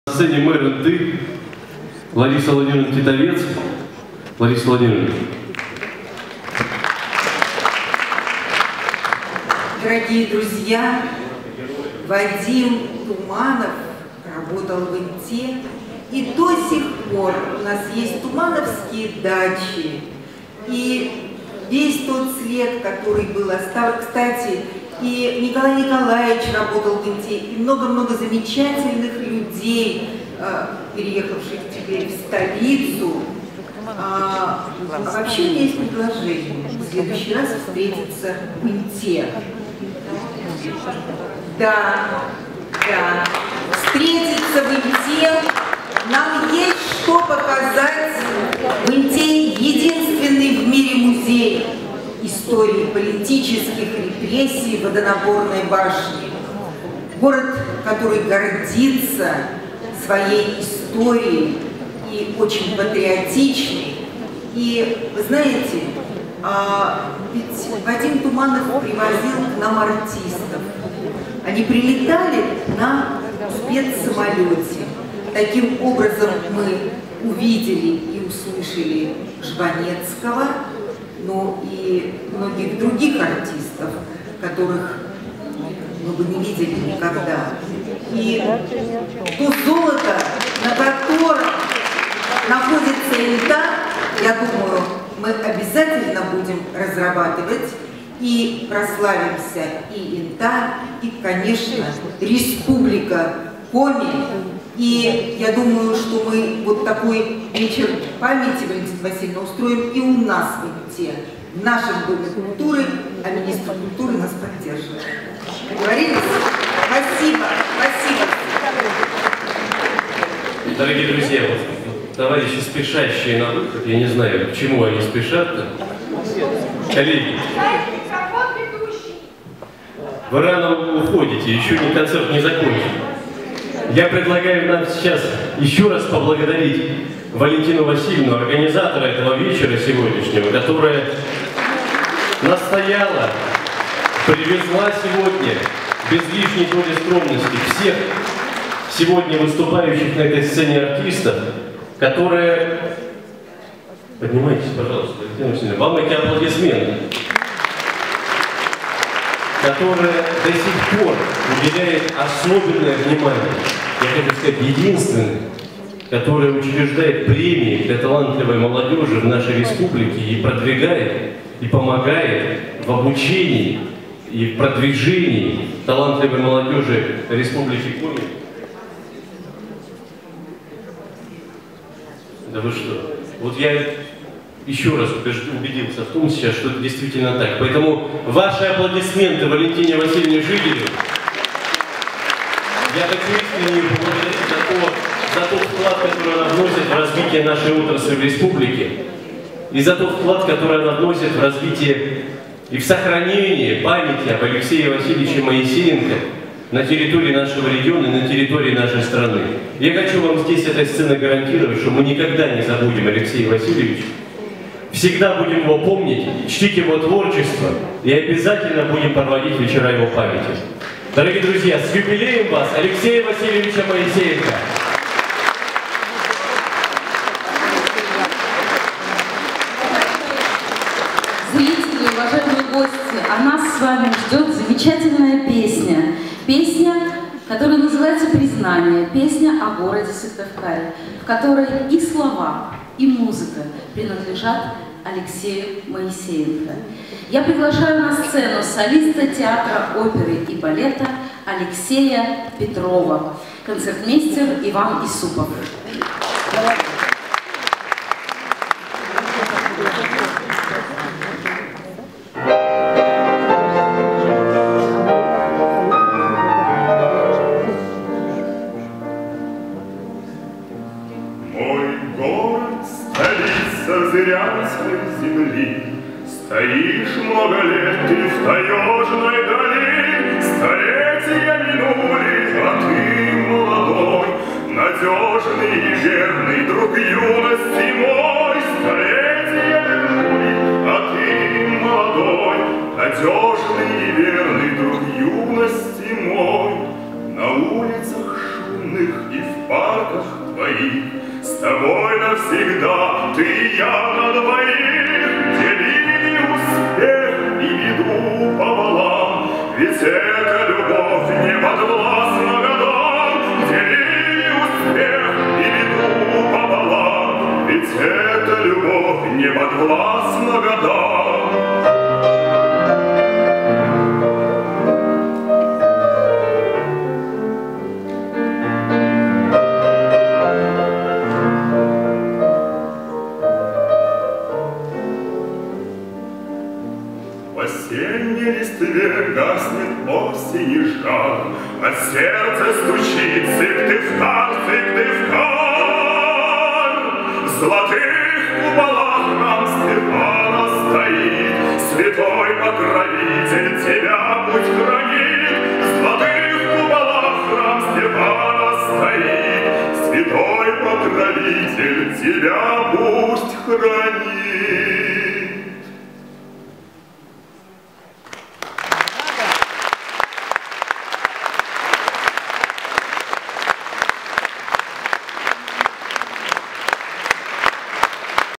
Вечер Памяти АЛЕКСЕЯ МОИССЕНКО, ч.12
Вечер Памяти  в День 70-летия Алексея Васильевича,  "МОИ КУМИРЫ" АЛЕКСЕЙ МОИСЕЕНКО,  зарисовка, ч.12,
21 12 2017, Коми республиканская филармония, г. Сыктывкар, Республика  Коми.